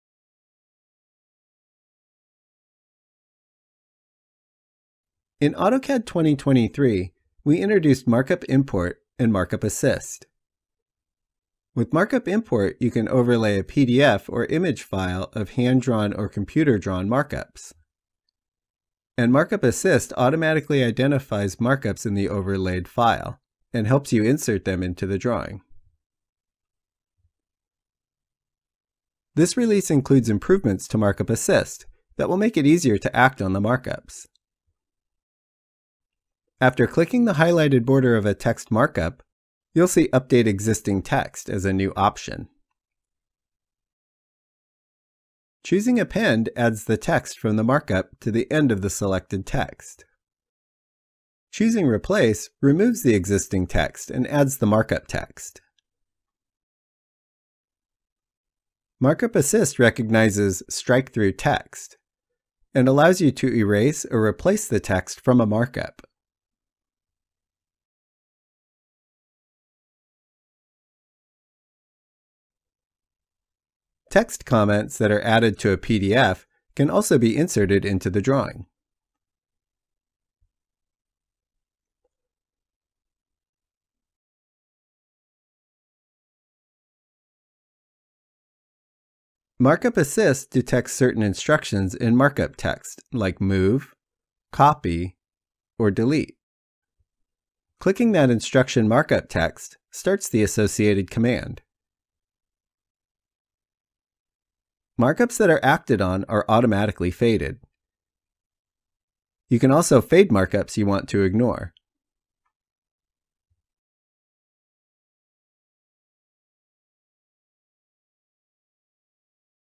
Markup Import and Markup Assist in AutoCAD 2024
Import feedback and incorporate changes to your drawings automatically when collaborating with teammates and reviewers, without additional drawing steps.